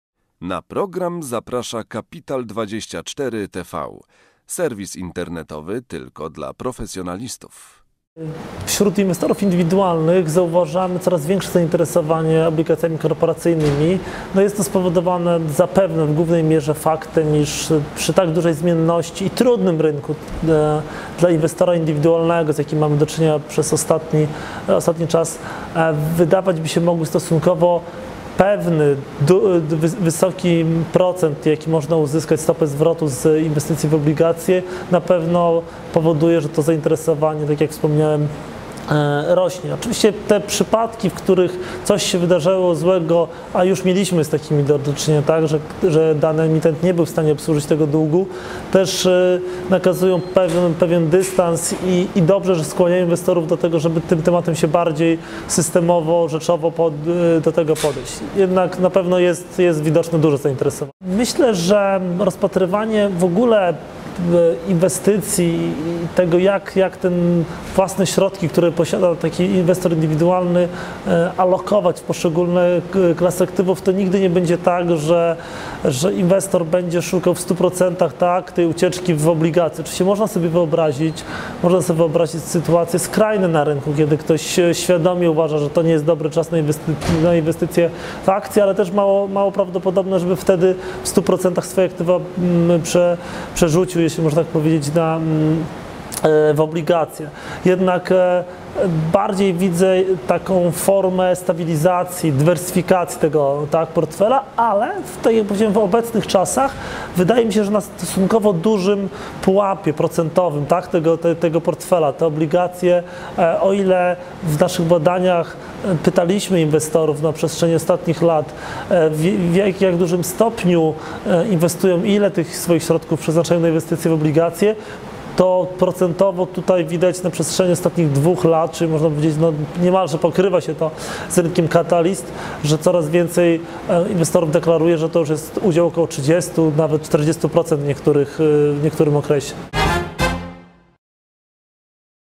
Inwestorzy indywidualni coraz częściej kupują obligacje
Komentuje: Jarosław Dominiak, prezes SII.